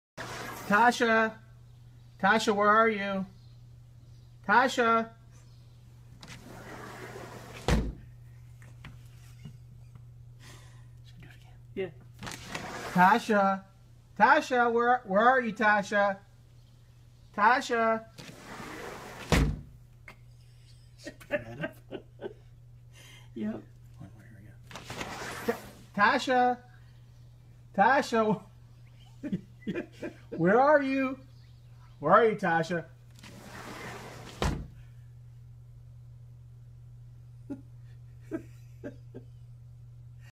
tasha where are you? Tasha hides from owner :)
Cheeky dog plays hilarious game of hide and seek with owner after refusing to come inside :)